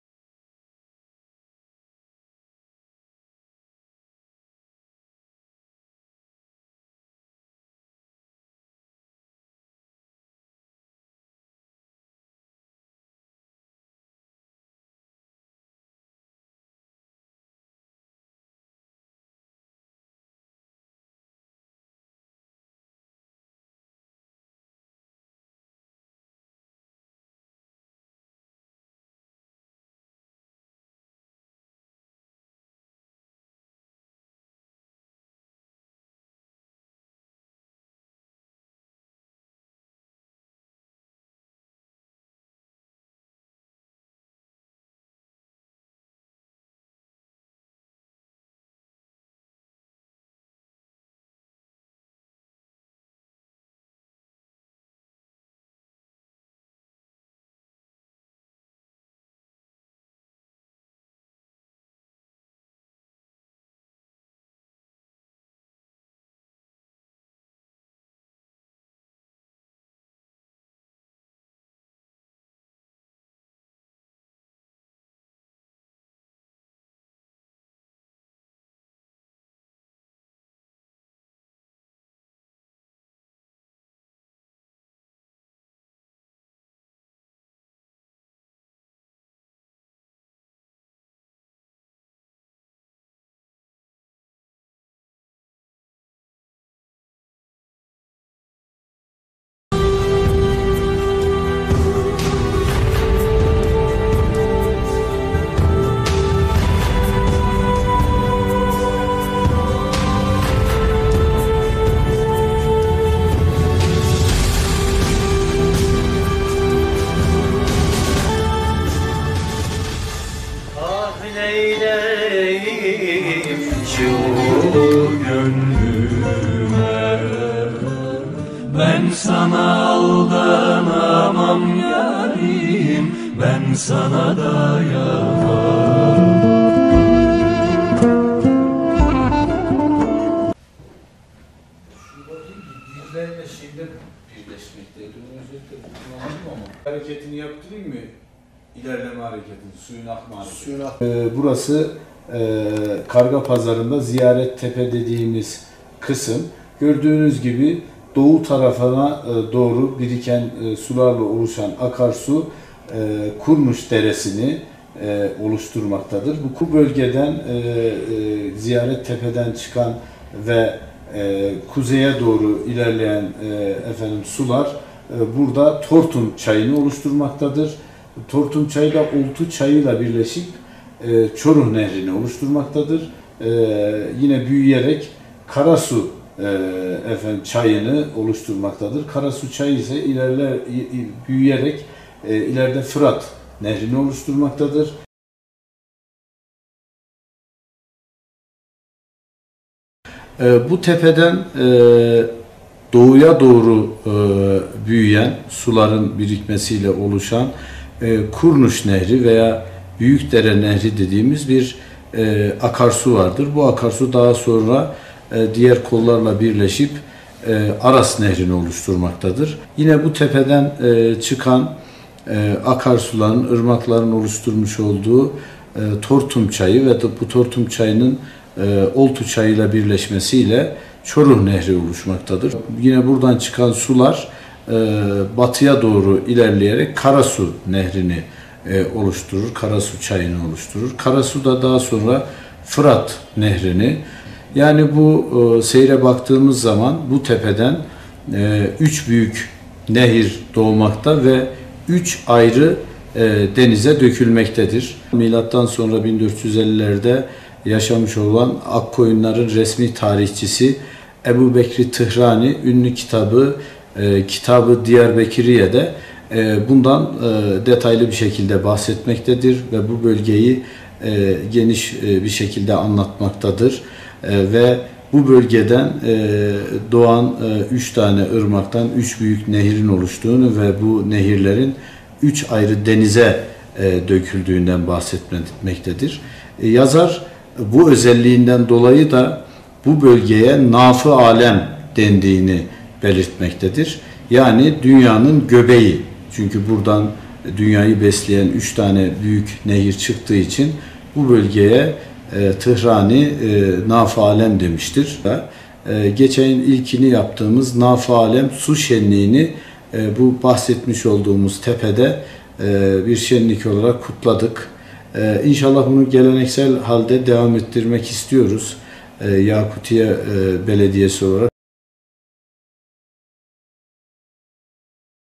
ERZURUM Dünyanın göbeği; Aras, Çoruh ve Fırat’ın doğduğu yer (VİDEO EKLENDİ)
Salih TEKİN/ ERZURUM, (DHA)- ERZURUM’un kuzeydoğusunda Kargapazarı Dağı’nın üzerinde bulunan 3 bin metre yüksekliğindeki Ziyaret Tepe, Aras, Çoruh ve Fırat nehirlerinin doğduğu yer. Akkoyunlu Devleti tarihçilerinin ‘dünyanın göbeği’ manasına gelen naf-ı alem dedikleri Ziyaret Tepe’deki kar sularının ve kaynak sularının oluşturduğu irili ufaklı göletler, 3 farklı denize dökülen 3 büyük nehre kaynaklık yapıyor. \r\nErzurum’un Yakutiye ilçesinin kuzeydoğusunda, Dumlu ve Köşk köylerinin sırtındaki Dumlu Dağları’nda bulunan 3 bin rakımlı Ziyaret Tepe, irili ufaklı çok sayıda göl ve gölete ev sahipliği yapıyor. Kaynak suları açısından zengin olan tepeden çıkan sular büyüyerek nehirlere dönüşüyor. Bu tepeden kaynağını alıp, doğuya akan sular Kurnuç Deresi ismiyle Aras Nehri’nin kaynağını oluşturuyor. Türkiye sınırlarını geçen Aras Nehri, Hazar Denizi’ne dökülüyor. Tepenin kuzey tarafına doğru akan sular, Tortum Çayı'nı meydana getirip, Oltu Çayı ile birleşerek Çoruh Nehri adını alıyor. Türkiye’nin en hızlı akan nehri olarak bilinen Çoruh, Gürcistan sınırlarında Karadeniz'le buluşuyor. Ziyaret Tepe’nin batısından çıkarak Erzurum Ovası’nda ‘Karasu’ adını alan akarsu, Erzincan’da ‘Fırat’ ismini alarak Basra Körfezi’ne dökülüp oradan da Umman Denizi’ne karışıyor. \r\n'TEPEDE ÇOK SAYIDA GÖL VE GÖLET VAR'\r\nTarihi boyunca birçok medeniyete ev sahipliği yapan Erzurum’un tatlı su kaynakları bakımından zengin bir kent olduğunu belirten Yakutiye Belediye Başkanı Mahmut Uçar, “Erzurum'un doğusunda Ziyaret Tepe dediğimiz yaklaşık 3 bin metre yüksekliğinde bir tepe var.
 Bu tepe üç büyük nehrin doğmuş olduğu yerdir. Bu tepede kar suyu ile kaynak sularının oluşturduğu çok sayıda su birikintisi, göl ve gölet var. Bu tepeden kaynak alan ve doğuya doğru giden sular Kurnuç Deresi'ni oluşturuyor. Bu dere daha sonra diğer kollarla birleşip Aras Nehri’ne dönüşüyor. Aras Nehri de Hazar Denizi’ne dökülüyor. Yine bu tepeden çıkan suların kaynak olduğu Tortum Çayı da Oltu Çayı ile birleşerek Karadeniz’e dökülen Çoruh Nehri’ni oluşturuyor. Bu bölgenin batısından çıkan sular Karasu Nehri’ne kaynaklık yapıyor. Karasu daha sonra Fırat Nehri ismini alarak daha ileride Dicle ile birleşerek Basra Körfezi’ne dökülüyor. Yani bu bölgeye baktığımız zaman bu tepeden üç büyük nehir doğmakta ve 3 ayrı denize dökülmektedir” diye konuştu.\r\nKİTAB-I DİYARBEKRİYYE'DE \r\nAkkoyunlu Devleti’nin resmi tarihçisi Ebu Bekr-i Tihrani’nin 1470’li yıllarda yazdığı ‘Kitab-ı Diyarbekriyye’ adlı kitabında bölgeyle ilgili detaylı değerlendirmelerde bulunduğunu kaydeden Başkan Uçar, “Bölgeyle ilgili durum tarih kaynaklarında da yer alıyor. Özellikle bölgede hüküm süren Akkoyunluların resmi tarihçisi Ebu Bekr-i Tihrani, kaleme aldığı ‘Kitab-ı Diyarbekriyye’de bu bölgeden detaylı bir şekilde bahsederek bu bölgeyi geniş bir şekilde anlatmaktadır. Bölgeden doğan 3 büyük nehrin üç ayrı denize döküldüğünden bahsetmektedir. Yazar, nehirlere kaynaklık etmesi ve bu nehirlerin dünyanın önemli bir kısmını suladığından dolayı da bu bölgeye ‘naf-ı alem’ dendiğini belirtmektedir. Yani ‘dünyanın göbeği’. Çünkü buradan dünyayı besleyen üç tane büyük nehir çıktığı için bu bölgeye bu isim verilmiştir” dedi.\r\nDünyanın göbeği olarak ifade edilen bölgeyi tanıtmak için geçen yıl ‘Naf-ı Alem Su Şenlikleri ve Gençlik Kampı’nı organize ettiklerini belirten Mahmut Uçar, şenlikleri geleneksel hale getirmeyi planladıklarını söyledi. (DHA) \r\nFOTOĞRAFLI\r\n\r\n\r\n